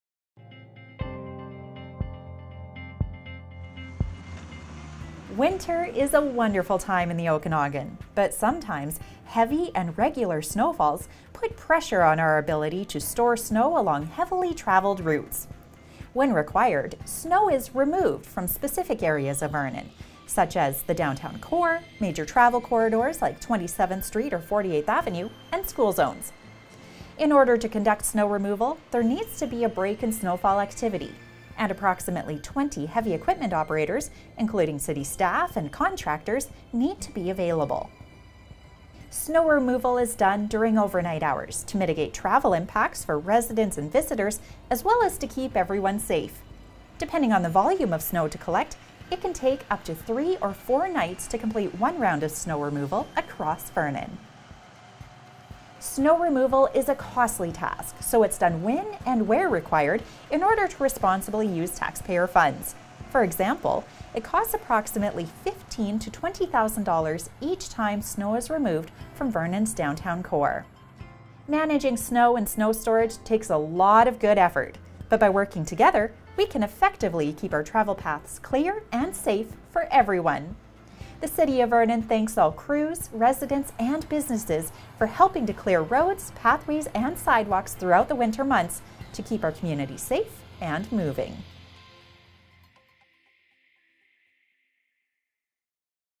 Snow removal in the City of Vernon
Winter is a wonderful time in the Okanagan, but sometimes heavy and regular snowfalls put pressure on our ability to store snow along heavily travelled routes in Vernon.

When required, snow is removed from specific areas of the city such as the downtown core, major travel corridors like 27th Street or 48th Avenue, and school zones. Here's a look at how it's done.